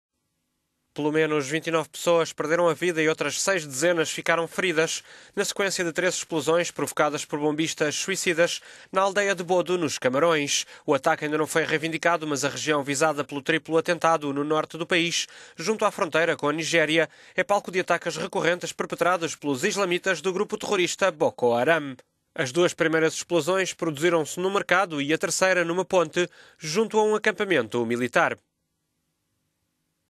Triplo atentado mortífero no norte dos Camarões
Pelo menos 29 pessoas perderam a vida e outras seis dezenas ficaram feridas, na sequência de três explosões, provocadas por bombistas suicidas, na aldeia de Bodo, nos Camarões.

O ataque ainda não foi reivindicado, mas a região visada pelo triplo atentado, no norte do país, junto à fronteira com a Nigéria, é palco de ataques recorrentes perpetrados pelos islamitas do grupo terrorista Boko Haram.